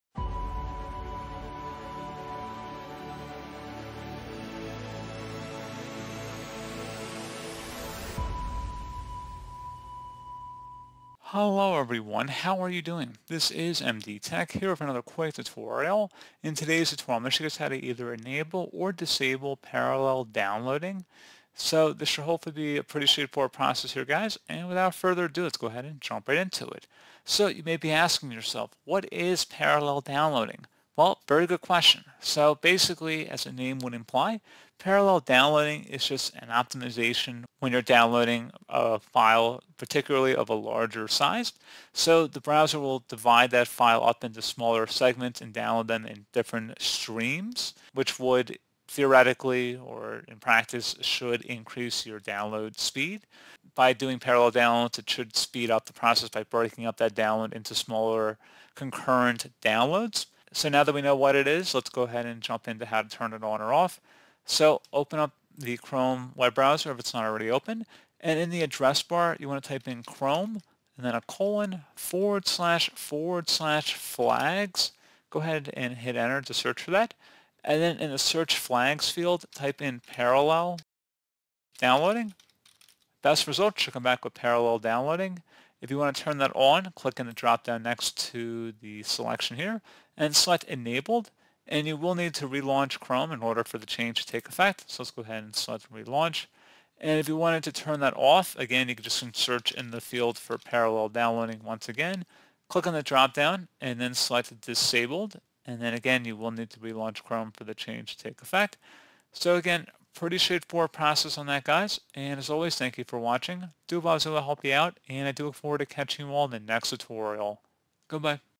How to Enable Parallel Downloading on Google Chrome [Guide]
Is your browser taking forever to download large files? If so, you can enable parallel downloading, which allows your browser to download a file in multiple streams, ultimately boosting the download speed. But what exactly is the parallel downloading feature, and how can you enable it in different browsers?

Chrome is the most popular browser for internet users. chrome is an open-source program for accessing. Chrome is stable and easy-to-use web browser however, it is not the fastest browser in the world or India. it has some issues with ram management and downloading speed. if you are facing slow downloading speed here is a method to increase download speed in Chrome are as follows.